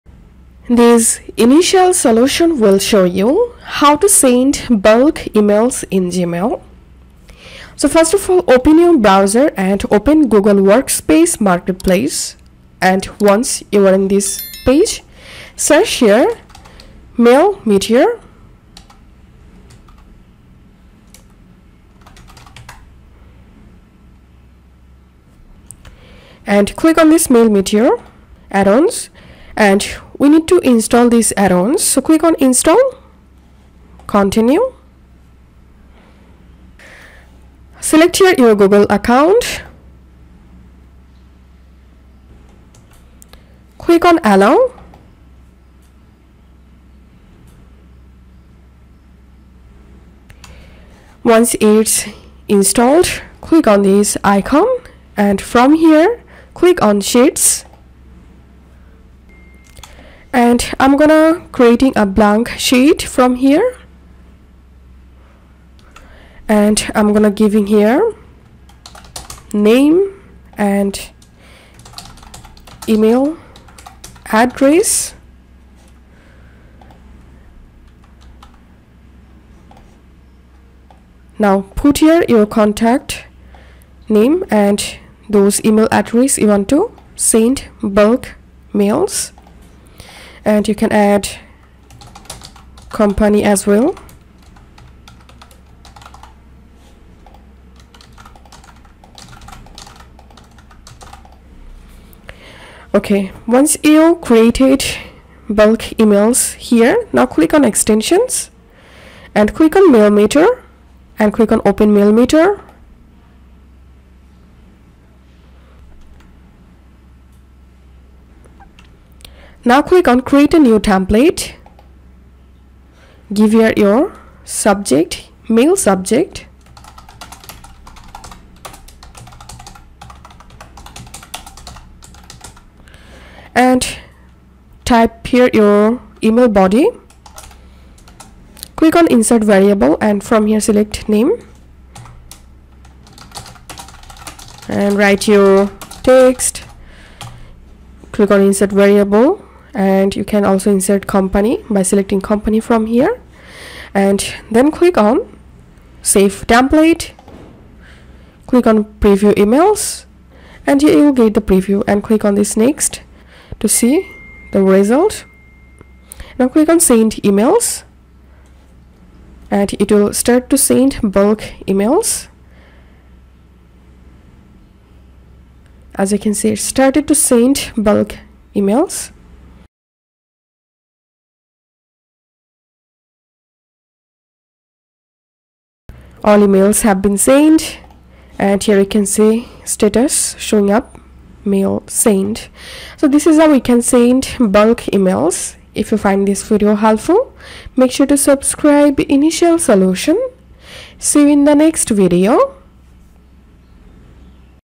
How to send bulk emails in Gmail 2024 | Initial Solution
Step 1: Install a Mail Merge extension.
Step 2: Draft your message.
Step 4: Start Mail Merge.
Step 5: Send Mass Email.